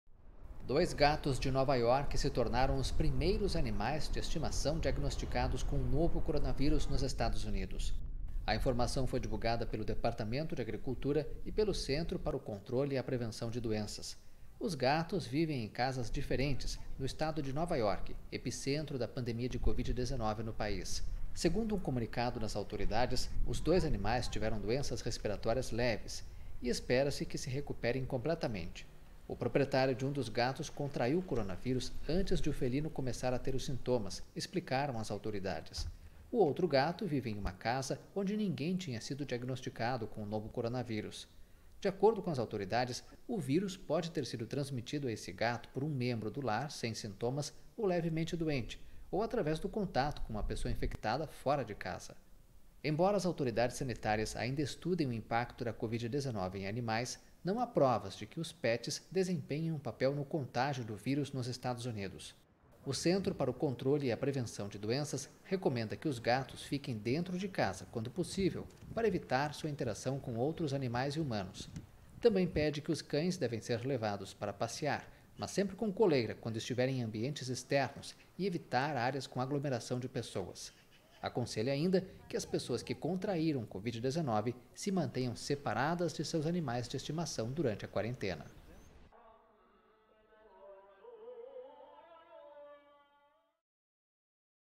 Gatos de NY com Covid-19 | AFP
Dois gatos de Nova York se tornaram os primeiros animais de estimação diagnosticados com o novo coronavírus nos Estados Unidos. A informação foi divulgada pelo Departamento de Agricultura e o Centro para o Controle e a Prevenção de Doenças. Os gatos vivem em casas diferentes no estado de Nova York, epicentro da pandemia de Covid-19 no país. Segundo um comunicado das autoridades, "os dois animais tiveram doenças respiratórias leves e se espera que se recuperem completamente". O proprietário de um dos gatos contraiu coronavírus antes de o felino começar a ter os sintomas, explicaram as autoridades. O outro gato vive em uma casa onde ninguém tinha sido diagnosticado com o novo coronavírus. De acordo com as autoridades, "o vírus pode ter sido transmitido a esse gato por um membro do lar sem sintomas ou levemente doente, ou através do contato com uma pessoa infectada fora de casa". Embora as autoridades sanitárias ainda estudem o impacto da COVID-19 em animais, "não há provas de que os pets desempenhem um papel no contágio do vírus nos Estados Unidos". O Centro para o Controle e a Prevenção de Doenças recomenda que os gatos fiquem dentro de casa quando possível para evitar sua interação com outros animais e humanos. Também pede que os cães devem ser levados para passear, mas sempre com coleira quando estiverem em ambientes externos e evitar áreas com aglomeração de pessoas. Aconselha, ainda, que as pessoas que contraíram COVID-19 se mantenham separadas de seus animais de estimação durante a quarentena.